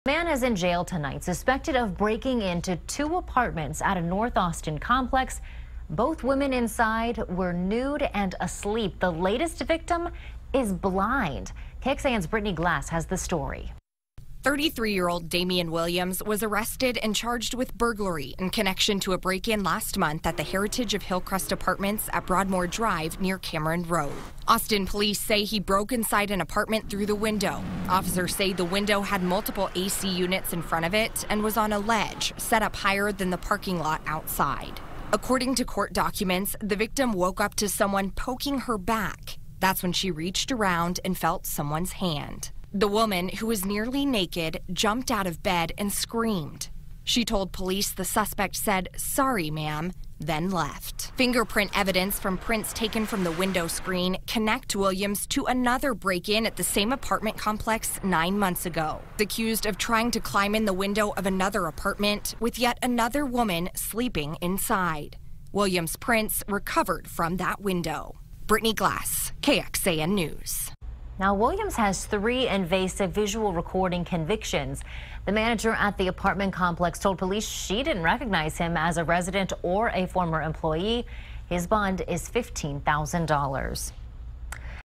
Blind woman woke to suspect touching her in north Austin break-in
Damien Williams was arrested and charged with burglary in connection to a break-in on March 11 at a north Austin apartment complex.